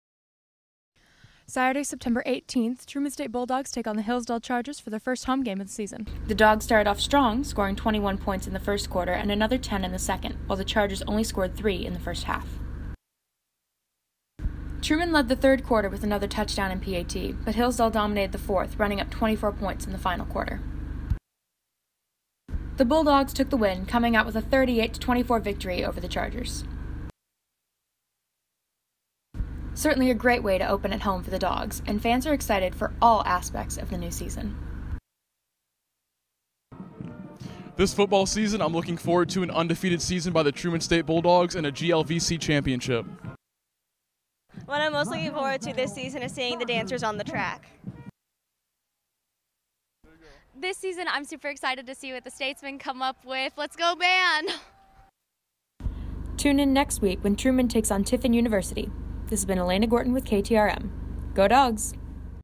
Truman Football's First Home Game (9/23/21)
Join Elaina Gorton as she reports on Trumans first home game of the season.